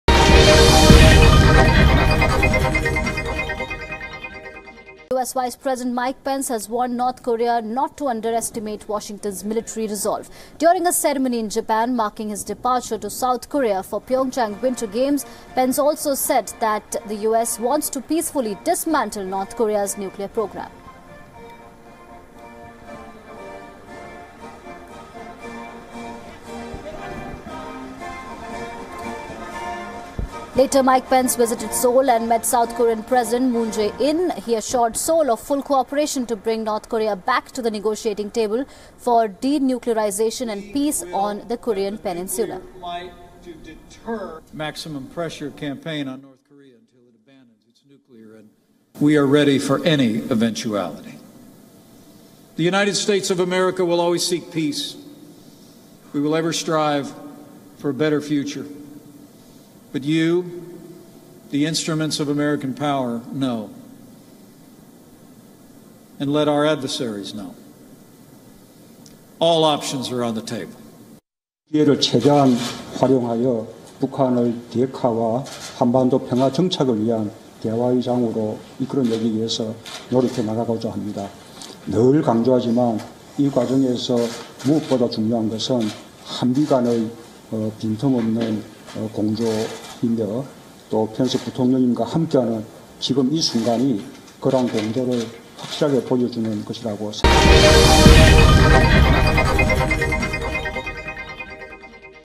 US Vice President warns North Korea
US Vice President Mike Pence said the United States wants to peacefully dismantle North Korea’s nuclear program but warned Pyongyang not to underestimate US military strength or resolve.